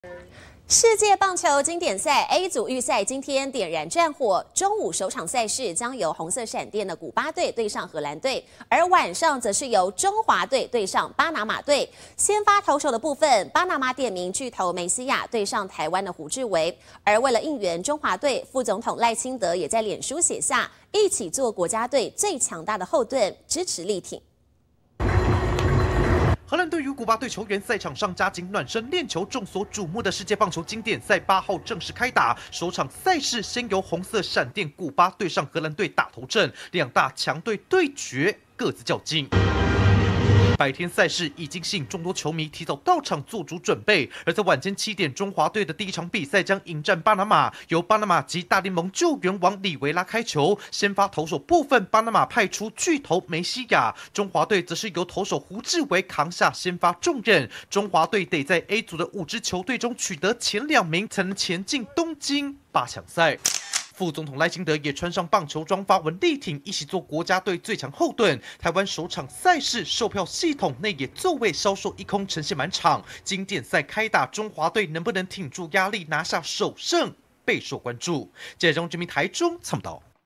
WBC中華隊首戰巴拿馬 胡智爲「大哥」掛帥先發
世界棒球經典賽A組預賽，今天(8號)點燃戰火，中午首場賽事，將由紅色閃電的古巴隊，對上荷蘭隊，晚間則是由中華隊對上巴拿馬隊，先發投手部分，巴拿馬點名巨投梅西亞，對上台灣的胡智為，而為了應援中華隊，副總統賴清德也在臉書寫下「一起做國家隊最強大的後盾！」支持力挺！